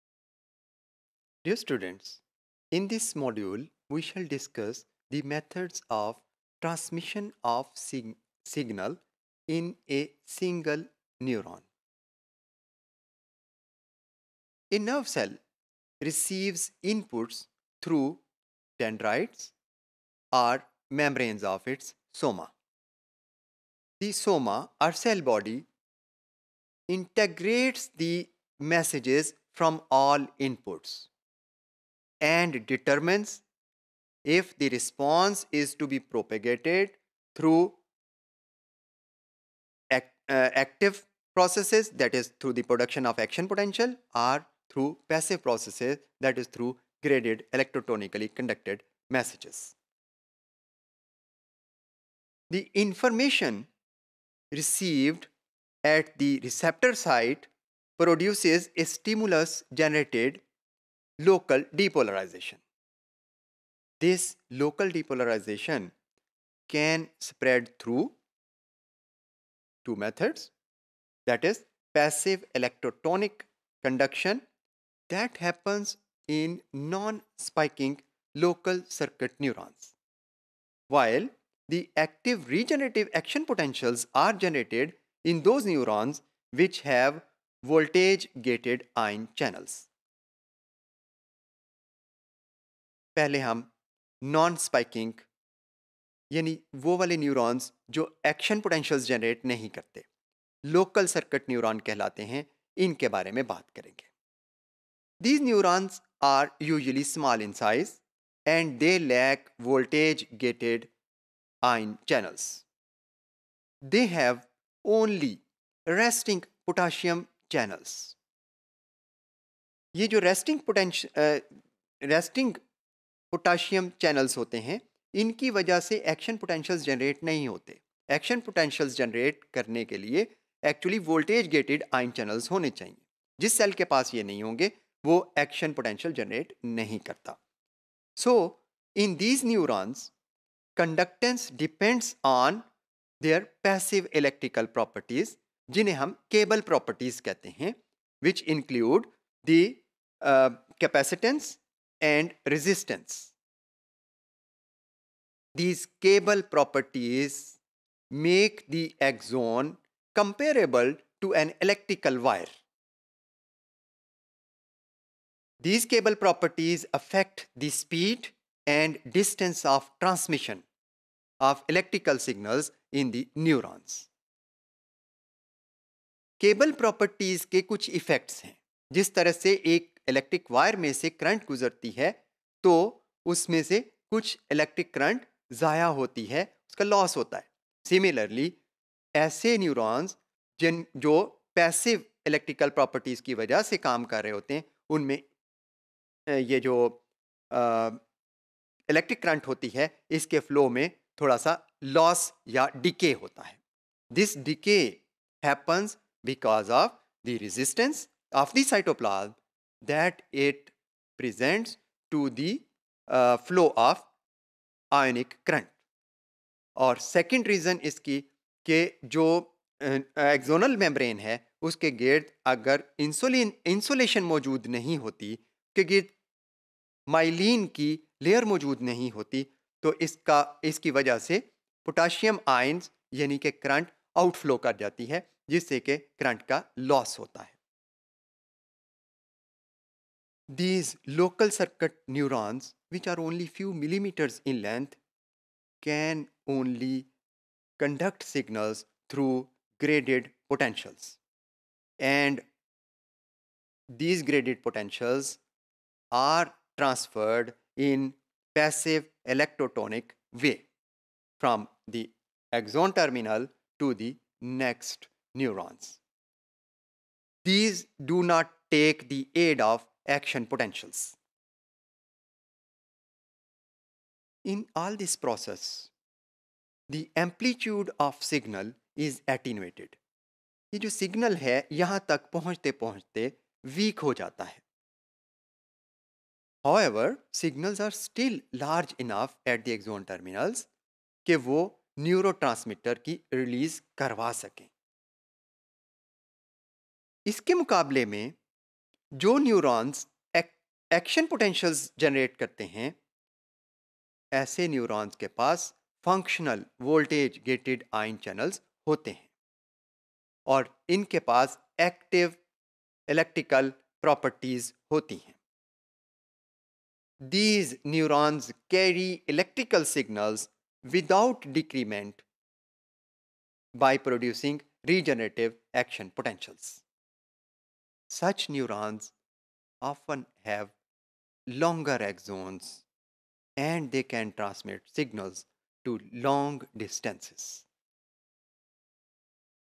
Transmission of Signals in a Single Neuron | Animal Physiology and Behavior | ZOO502T_Topic027
Topic027: Transmission of Signals in a Single Neuron,
By Dr. Ateeq ul Jaleel